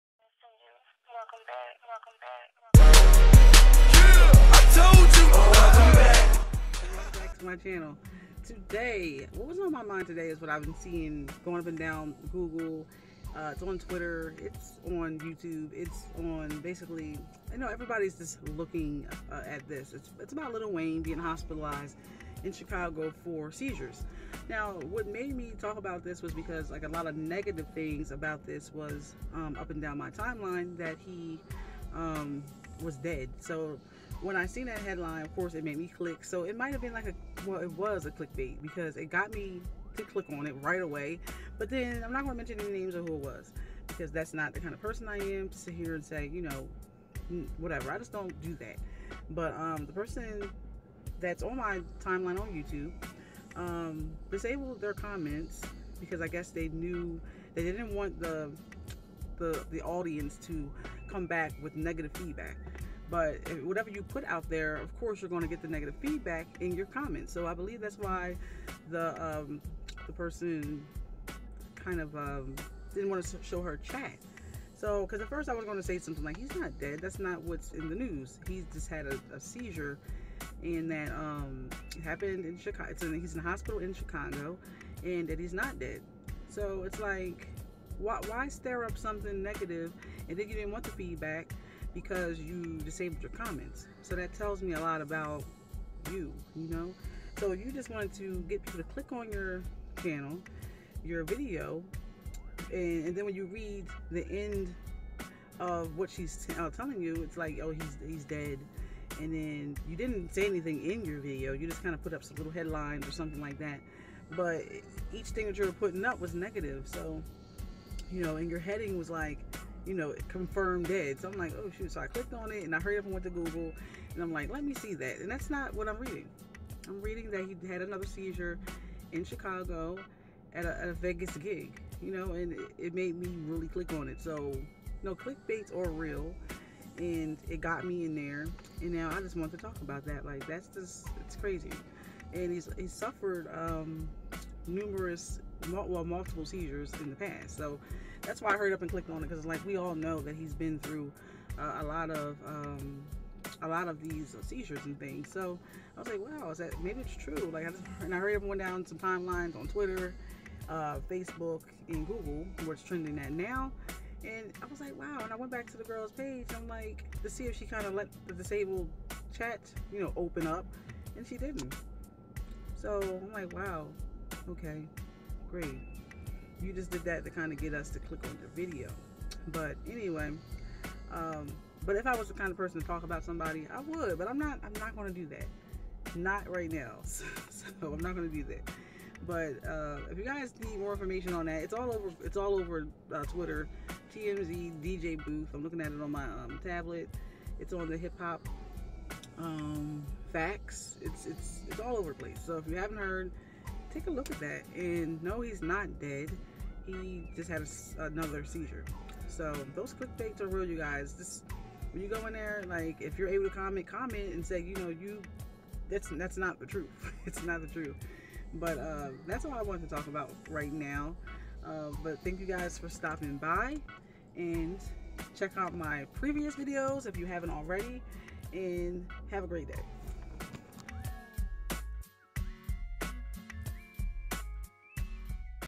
LIL WAYNE HAD ANOTHER SEIZURE
Lil Wayne had another SEIZURE ..but why do people make shit up, saying CONFIRMED DEAD...who confirmed it ..LOL seriously, I should put you on blast .